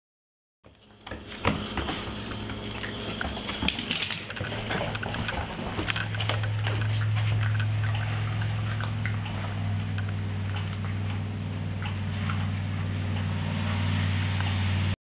sarah the dog 2
our minpin dancing for a treat